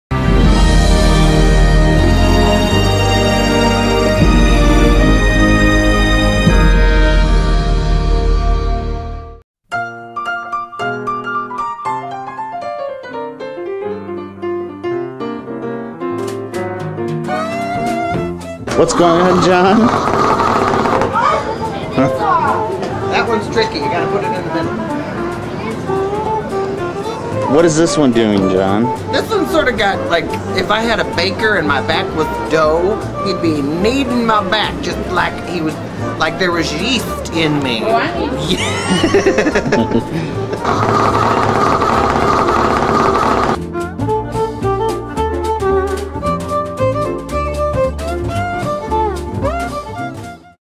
Visiting the Mall of America 4: "Yeast"
My friend describes the feeling of a vibrating lounge chair in the Sharper Image. Episode 4 of 7 in the Mall of America series.